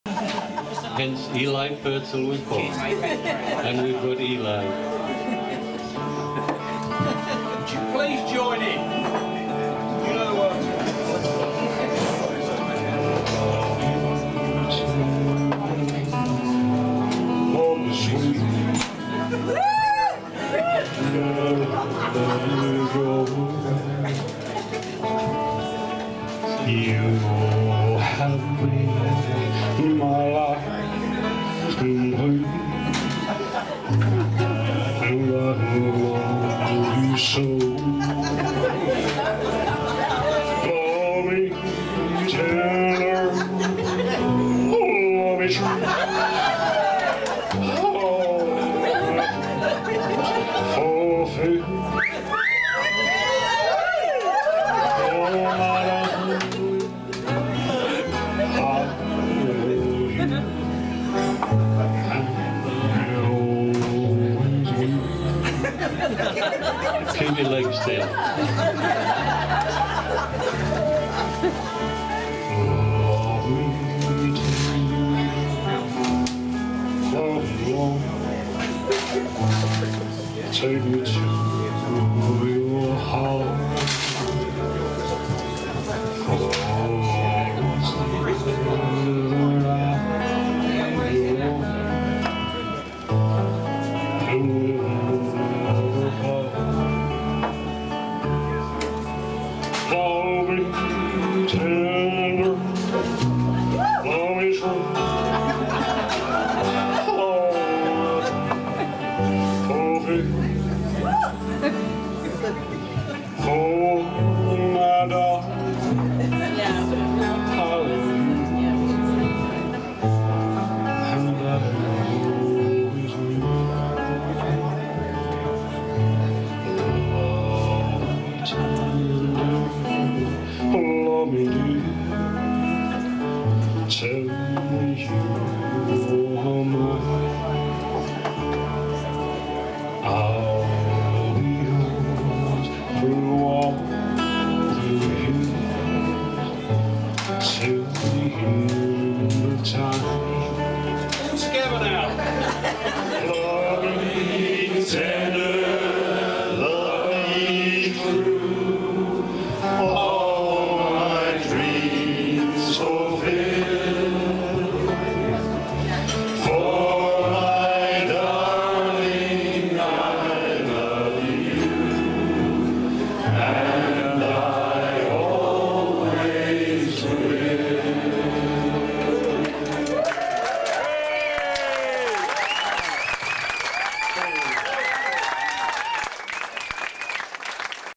Acid Reflux with Steve Savell, Gordon Westgarth & John Spaul
Lead singer Steve "Snake Hips" Savell of the legendary wrinkly rock band "Acid Reflux" perform at the "Shannon Express Chorus" Christmas party 2011.